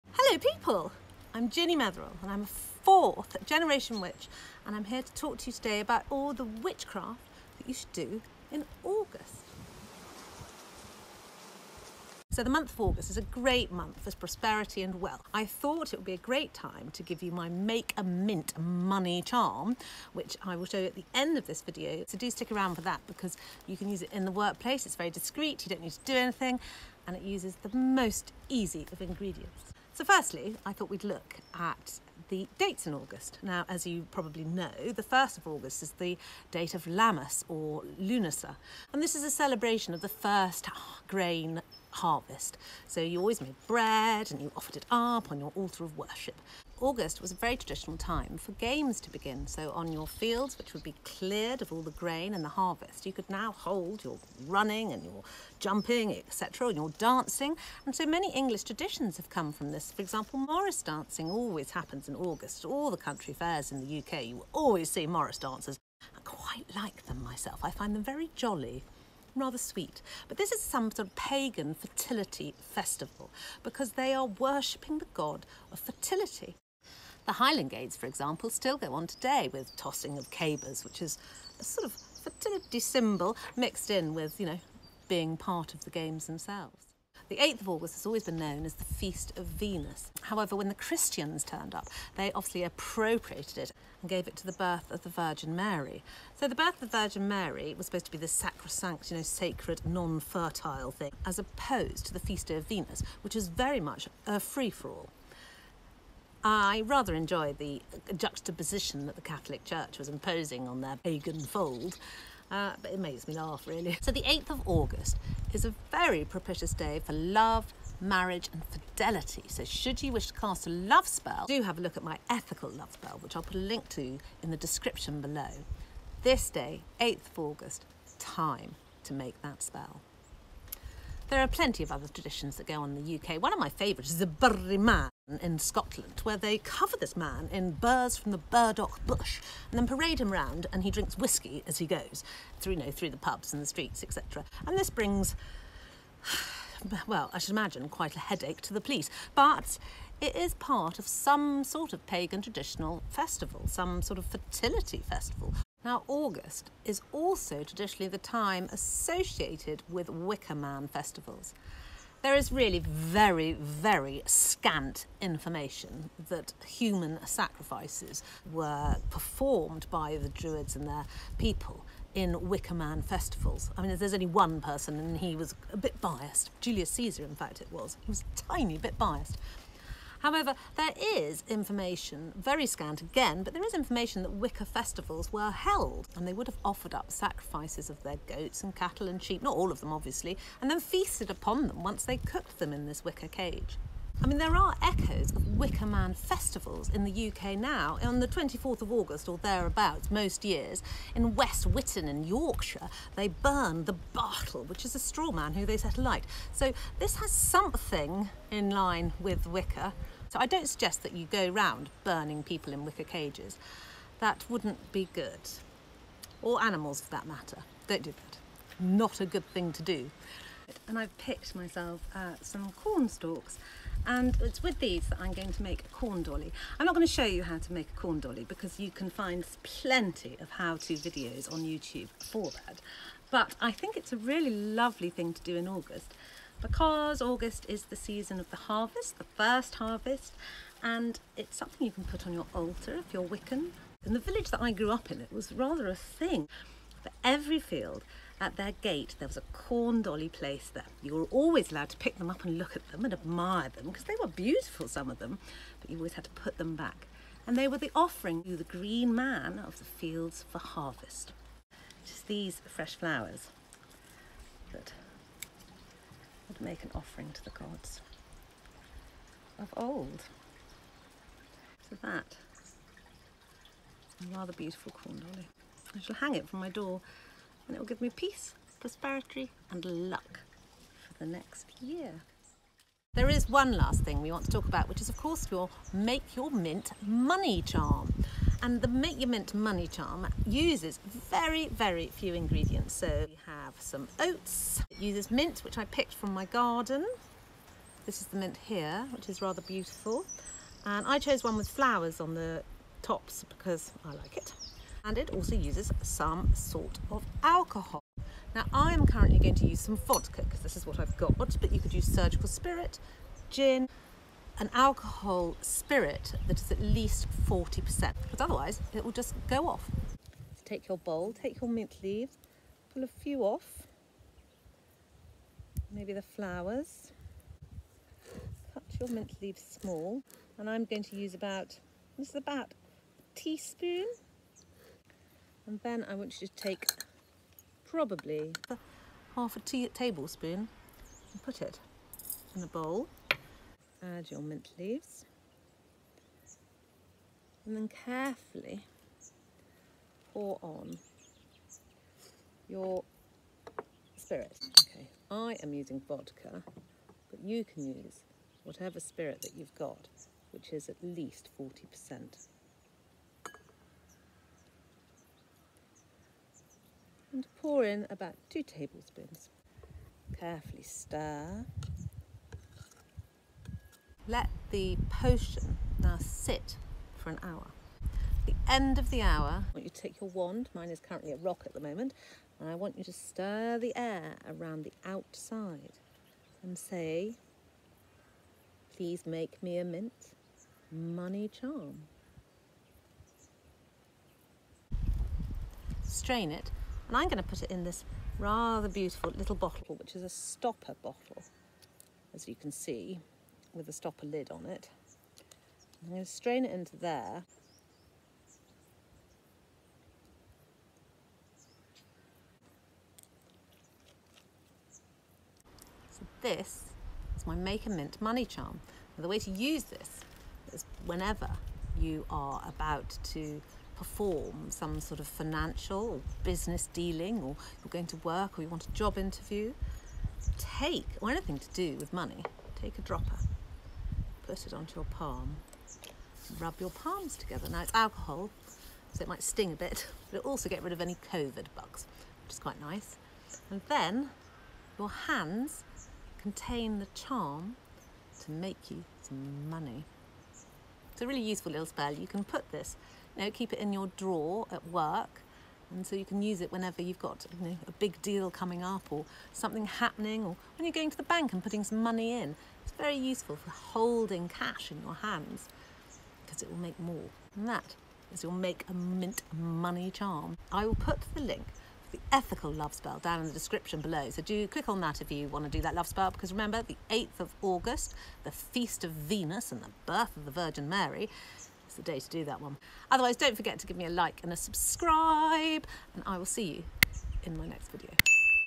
Witchcraft for August (Make a Mint Money Spell)
Witches have a lot of fun in August as the year is at its fullest and witchcraft can take so many differing paths.  It is a busy time of year and one that I enjoy the most.  Witchcraft has many aspects and this video is looking at traditional witchcraft and Druid/Wiccan rituals.  The time of year is strongly associated with the Green Man, Lammas or Lughnasadh and Wicker Man Festivals - Not all of these are something that I can recommend though so you will have to go your own path.

And for those looking for the Make a Mint Money Spell ingredients
1tsp Chopped Mint (dried mint is fine or even mint from a teabag)
2 tsps rolled Oats
2 tblsps Alcohol (minimum 40% Proof)

All these are available at your local store, but most of all ENJOY!
Ginny x

I would love to hear from you so please keep in touch x